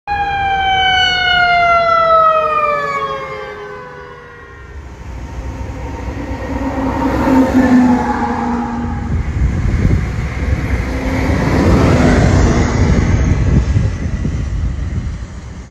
Womelsdorf Fire Co Brush Fire Response 8/2/24
Attack, Tanker, and Engine 47 responding to a brush fire on Norfolk Southern tracks in Heidelberg Twp.